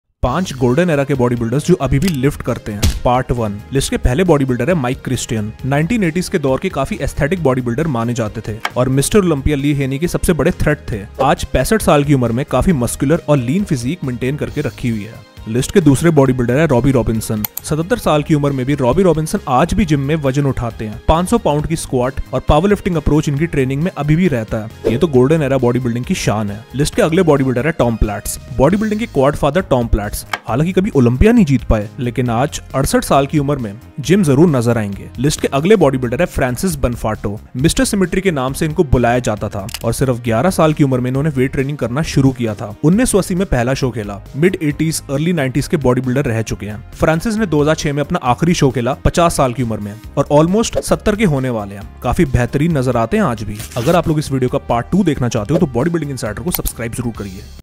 😲 आज भी BODYBUILDING करते है GOLDEN ERA वाले \\ Bodybuilders Who Still Lift
4 Golden Era bodybuilders who still lift to this day, bodybuilders who belong to the golden era of bodybuilding in 1970s, 80s and 90s who still lift to this day. Bodybuilders then vs now all in bodybuilding